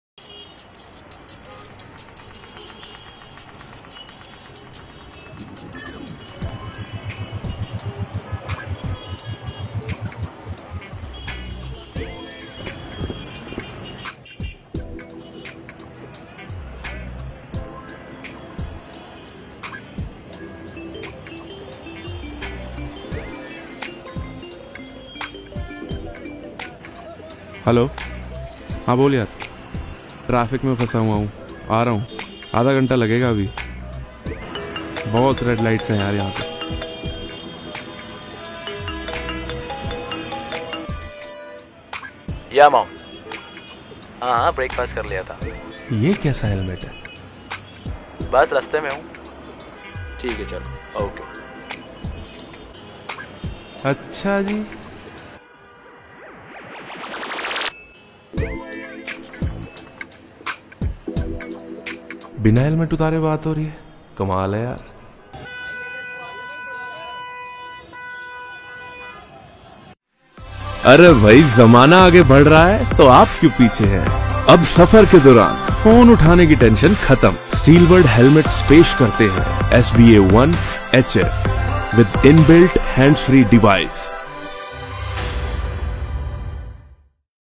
axor Apex Vivid Black Neon Yellow Helmet-L, Large
IMPS- Injection moulded Polycarbonate shell is used for High-Performance

Homologation - ECE R-22.05(Europe) & DOT FMVSS NO. 218(US) Helmet Safety Standards

Upper air intake vent is positioned to maximize air volume into the helmet interior, Back outlet vent is integrated with pc spoiler,
allowing it to be positioned for optimum flow-through ventilation
Optically correct visor, Quick and tooless visor change mechanism,
 Internal sun visor included
Back cushions Reflectors, Integrated Spoiler for Streamlined Presentation & Stability at High Speed,

 Tough Polycarbonate Material

Fit type: Universal Fit